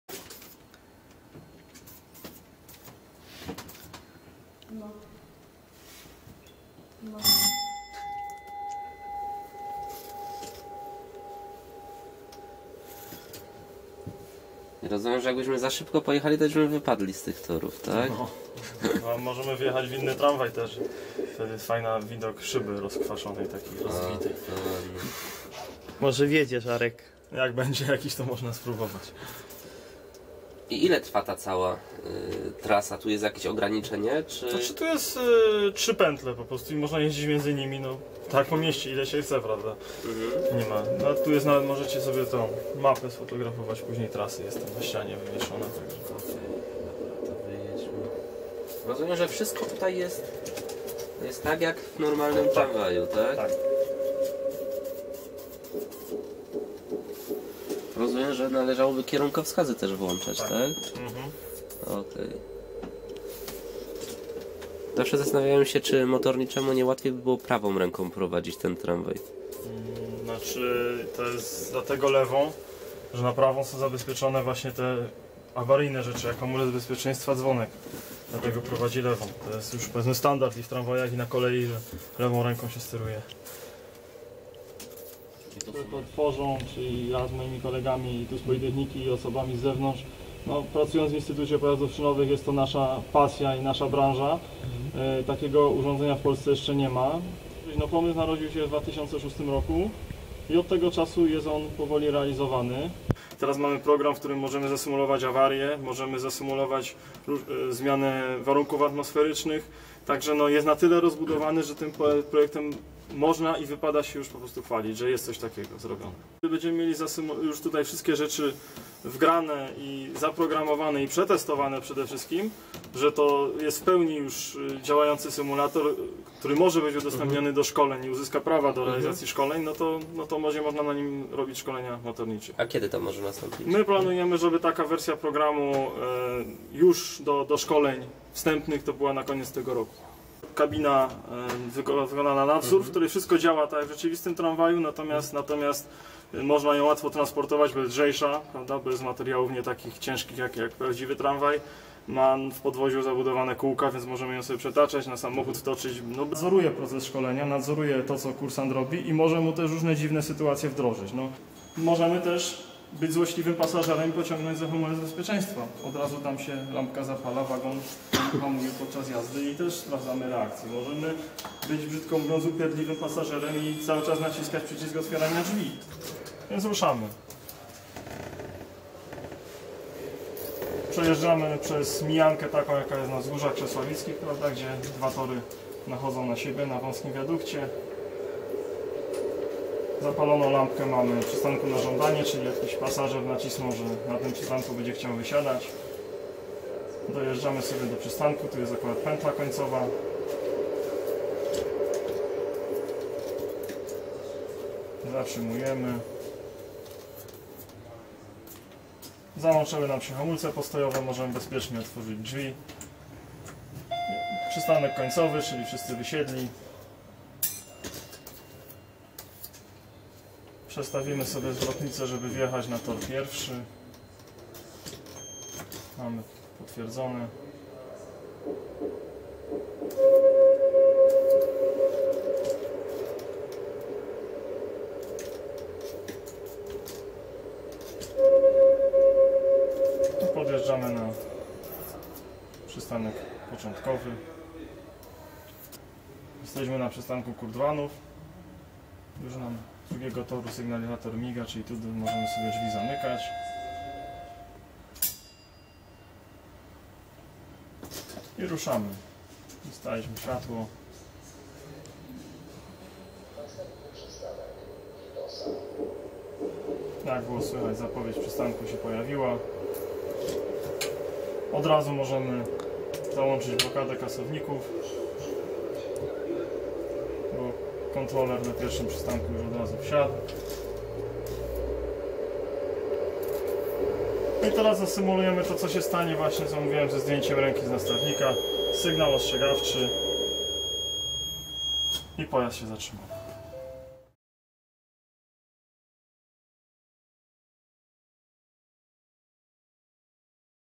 Kraków. Pierwszy w Polsce symulator tramwaju
Na Politechnice Krakowskiej powstał pierwszy w kraju symulator tramwaju. W programie odtworzono miejskie trasy tramwajowe wraz z przystankami. Projekt realizowany jest od 2006 r., wkrótce będą się mogli szkolić krakowscy motorniczowie.